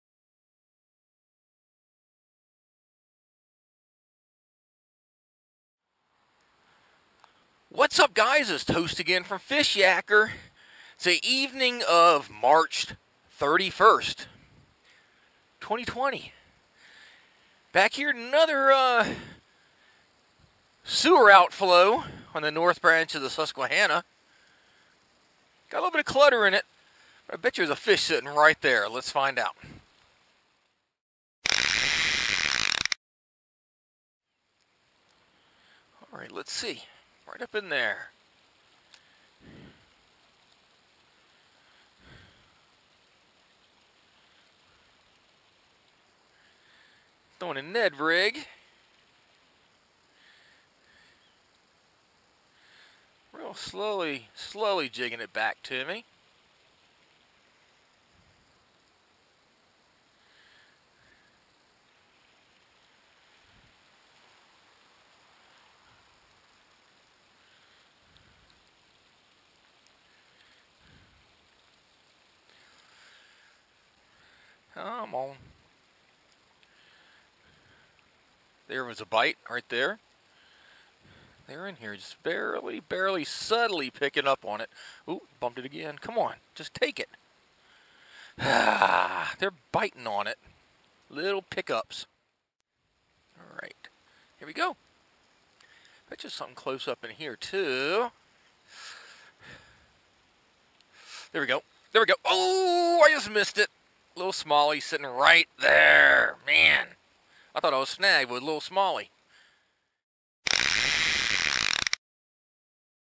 Susquehanna River High Water Outflow Fishing: 3/31/20
John "Toast" Oast from Fishyaker.com fishes the Susquehanna River near Bloomsburg, Pennsylvania. For more fishing and kayaking videos visit Fishyaker.com or the Fishyaker Youtube channel.